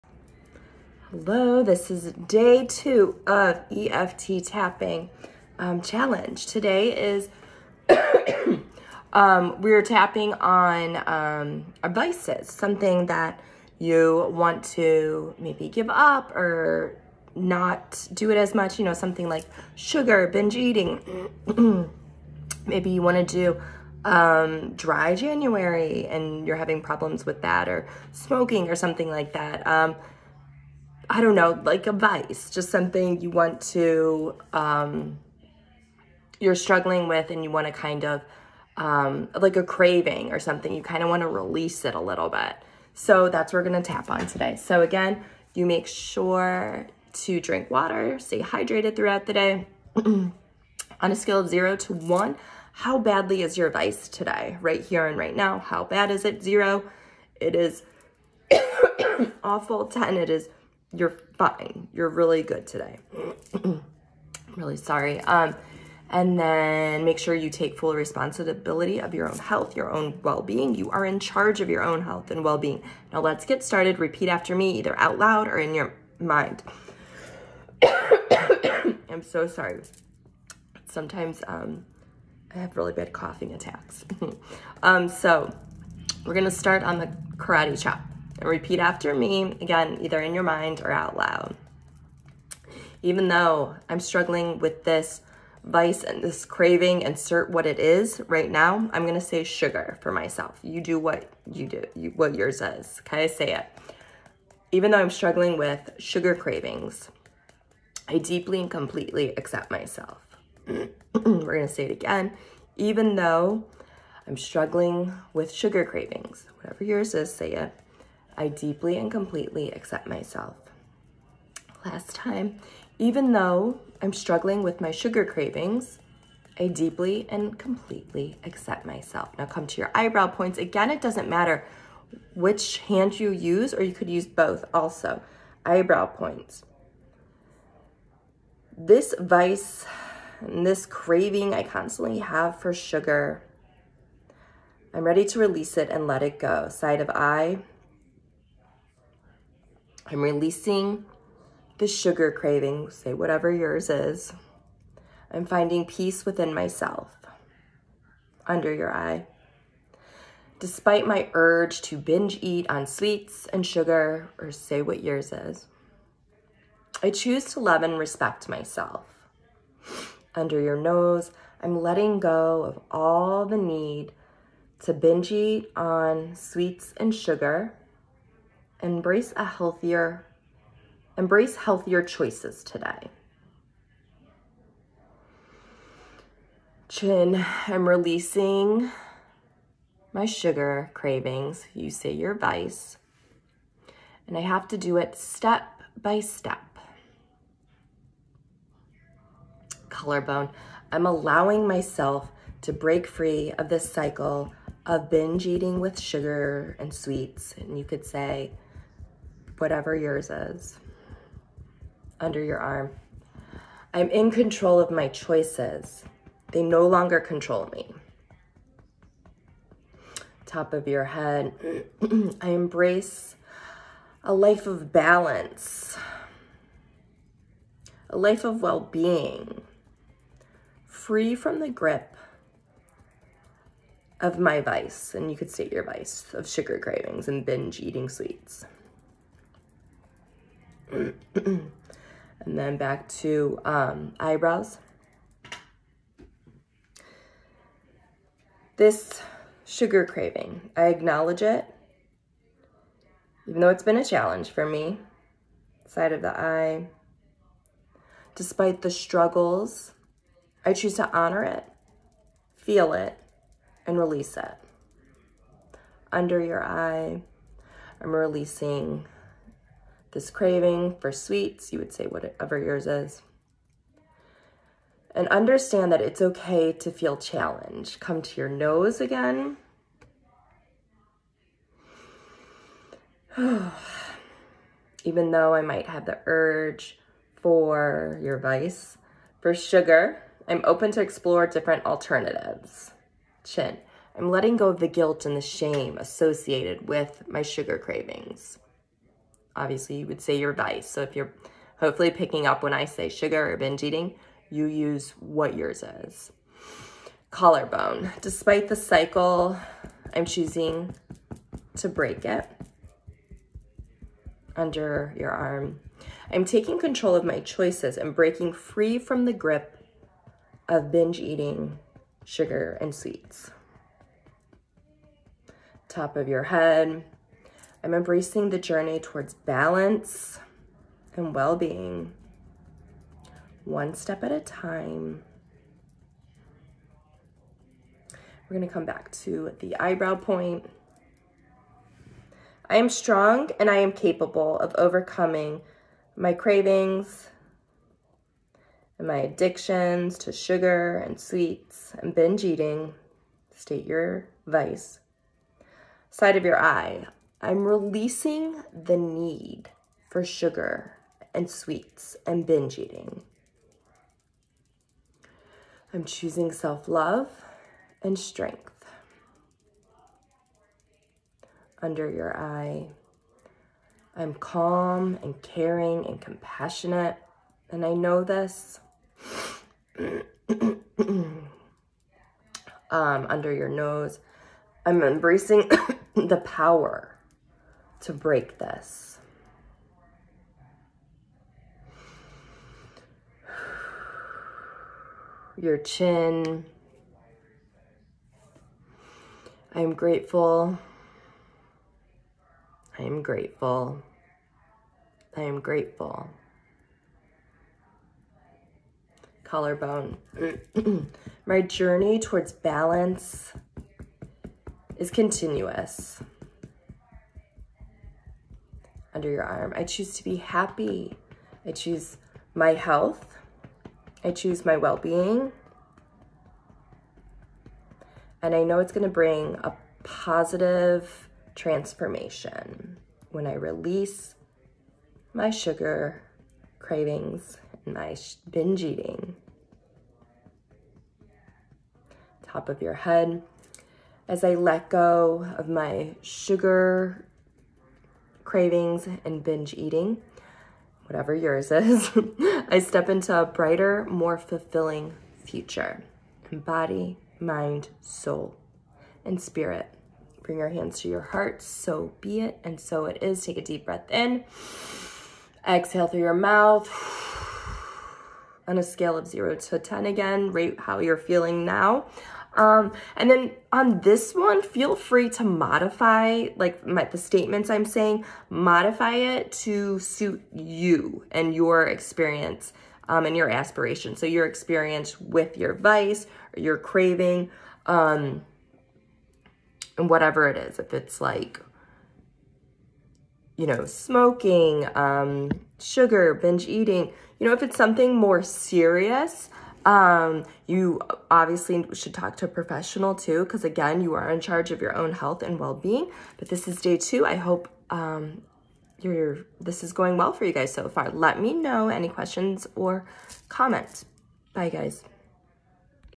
Day 2: EFT Releasing Your Vice
This is a tapping sequence for your vice. Whatever that is. It might be sweets, sugar, alcohol, smoking, binge eating, shopping.